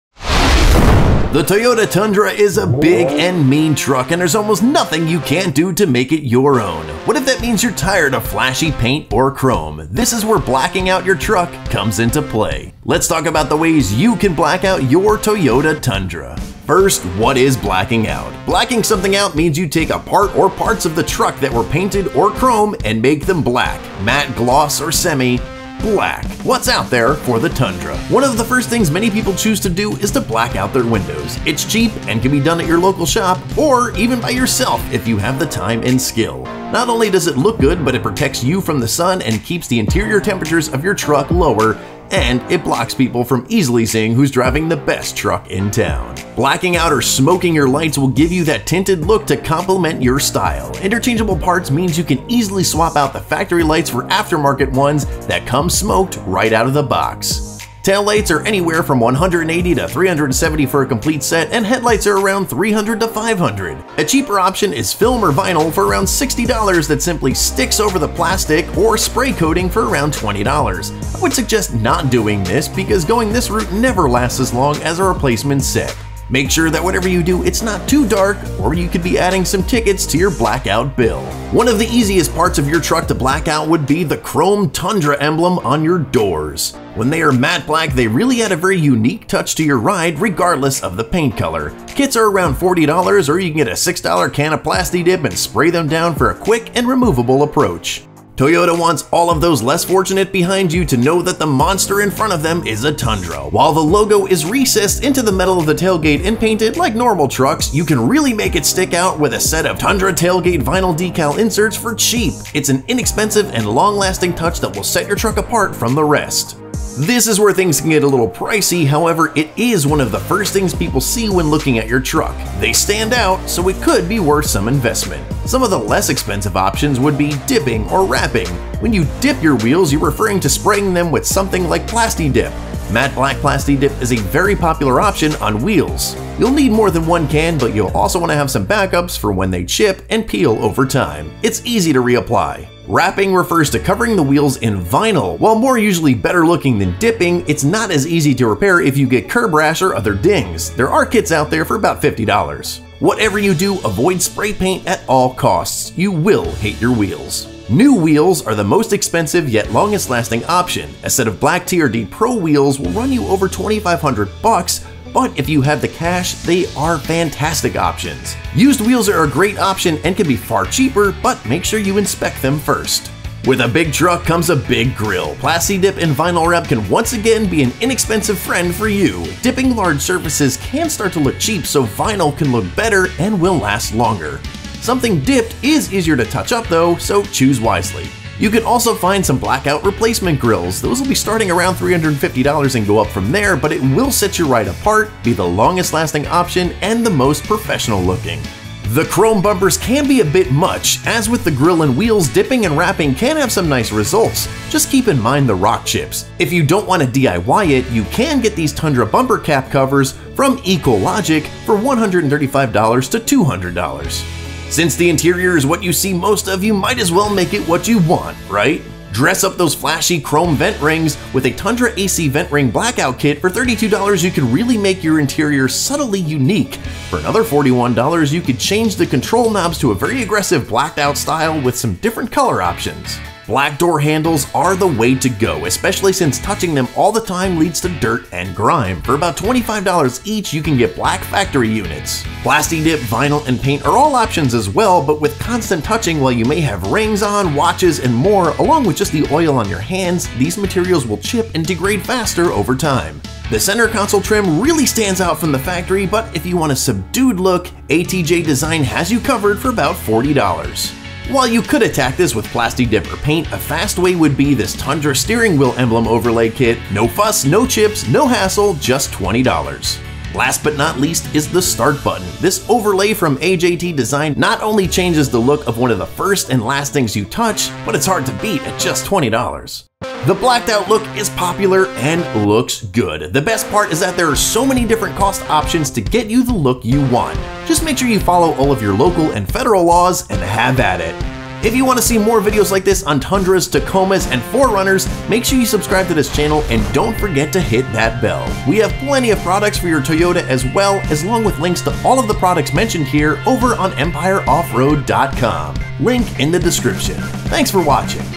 Ultimate Guide To Blacking Out Your Toyota Tundra
The Toyota Tundra is a big and mean truck, and there’s almost nothing you can’t do to make it your own. What if that means you’re tired of flashy paint and Chrome?  This is where blacking our your truck comes into play.  Let’s talk about the ways you can black out your Toyota Tundra.